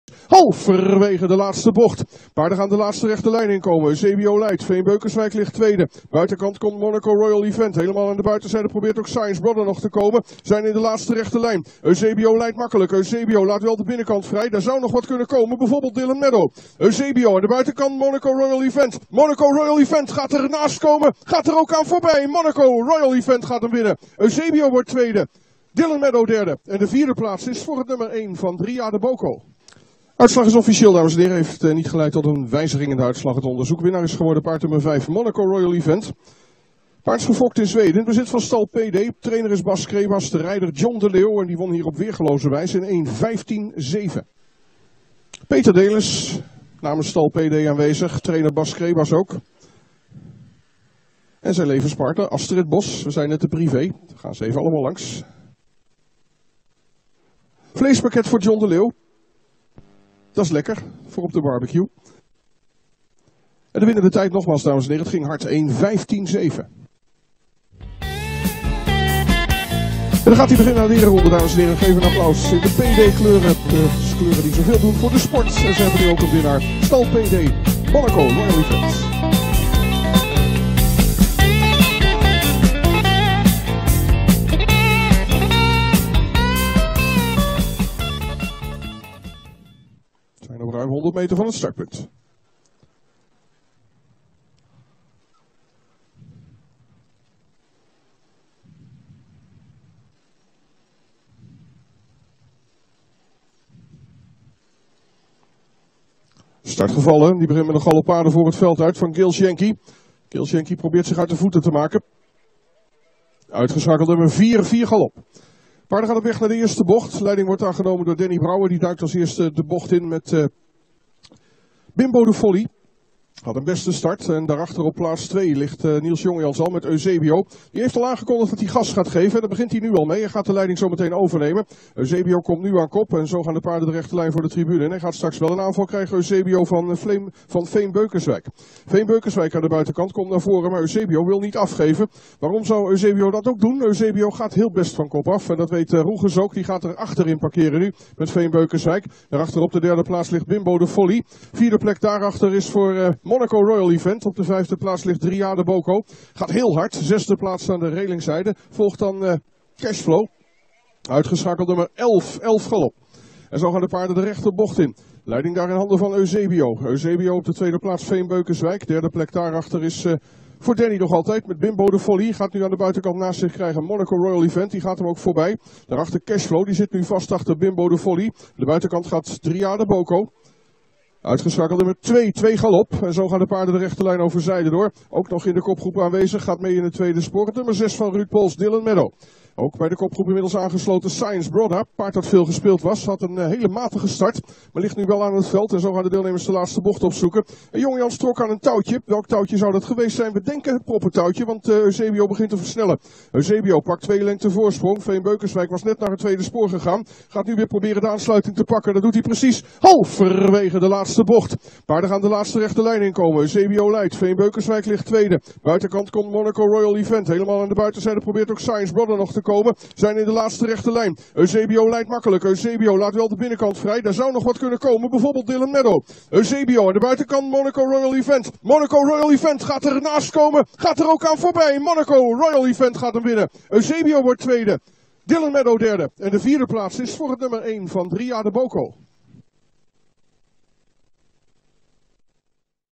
Monaco Royal Event & John de Leeuw winnen op Drafcentrum Stadspark Groningen 18 juni 2017
Monaco Royal Event & John de Leeuw winnen voor Payday Racing en tr. Bas Crebas "Omzet-prijs" op Drafcentrum Stadspark Groningen 18 juni 2017
Eig. combinatie Monaco Royal Event: Wilt Jan de Boer; José Delis & Jörgen Nygren; Astrid Bos & Bas Crebas; Stal Payday - Peter Delis
Gefokt in Zweden: Jürgen Hanke